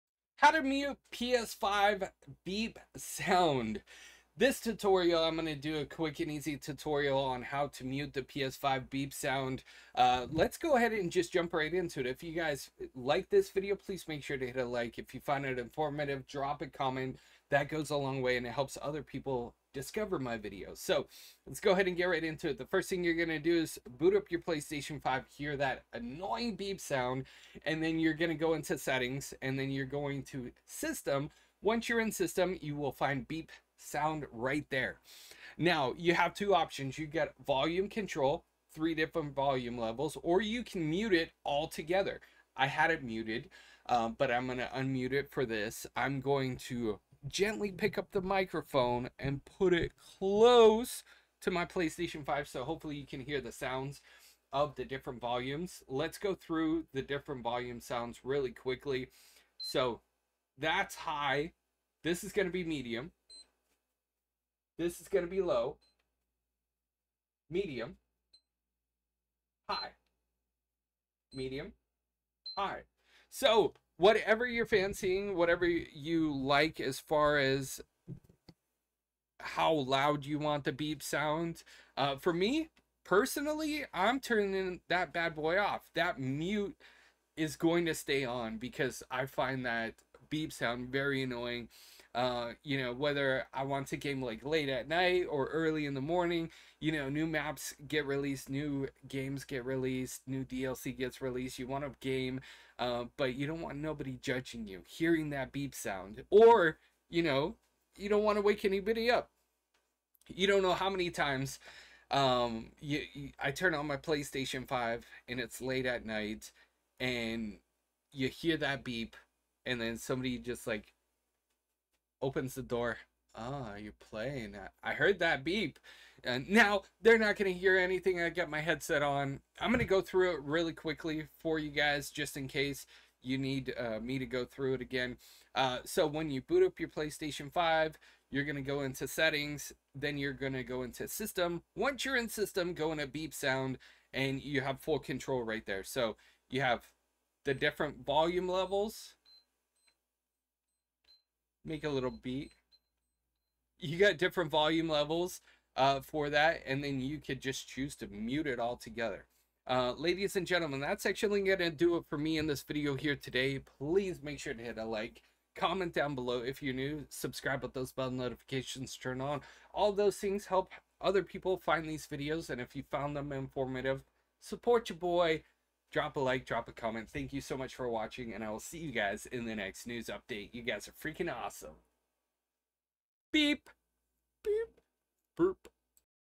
How to MUTE PS5 Beep (Turn OFF Beep Startup Sound)
Mute PS5 beep sound. You can now mute or adjust the volume of the PS5’s beep sound when turning it on or off, or putting it in rest mode.
Go to Settings - System - Beep Sound.
To adjust the beep sound volume, select [Volume].
To mute the beep sound, turn on [Mute Beep Sound].

Follow @MonkeyFlop on Twitter, Twitch, Facebook, or just search MonkeyFlop